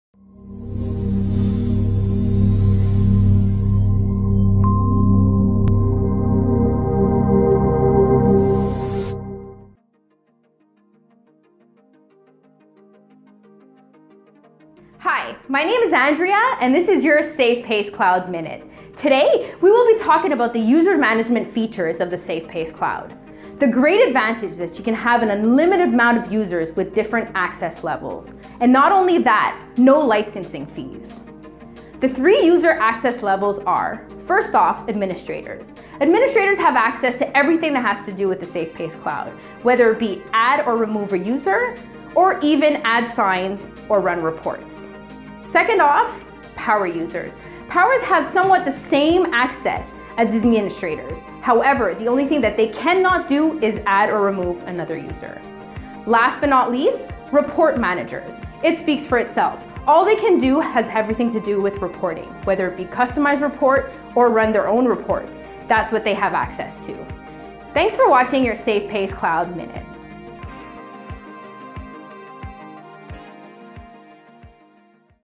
SP Cloud Minute - User Management
Wondering what you can do with SafePace Cloud? Or if you can assign different access limits to different users? Find out about the different kind of users you can assign in SafePace Cloud and what each can do.